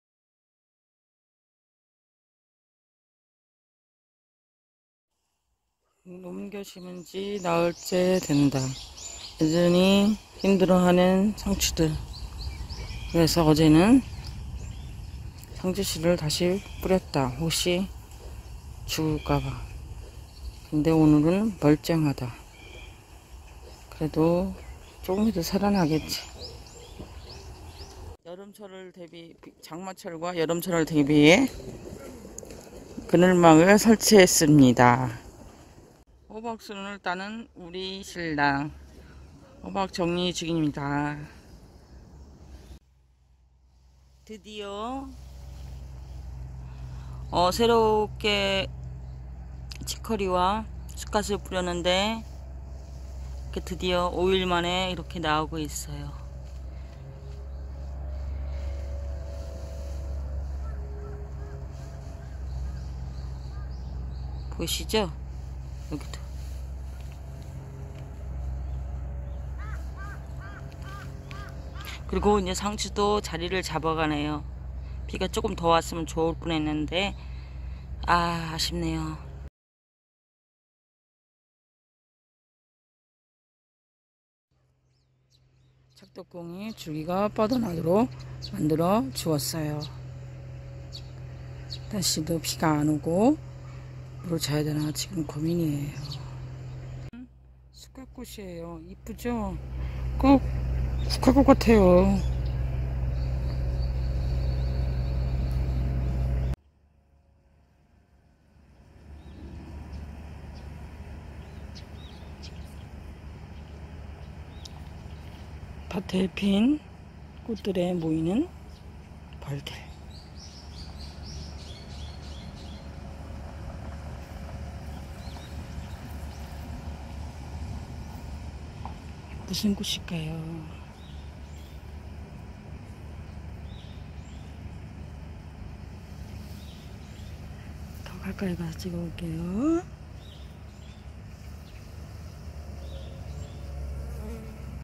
2019 6 마지막주 주말농장
더운 날씨 너무 가무네요
날마다 씨뿌린곳과 새로이 옮겨 심은 곳에 물을 주고 있어요.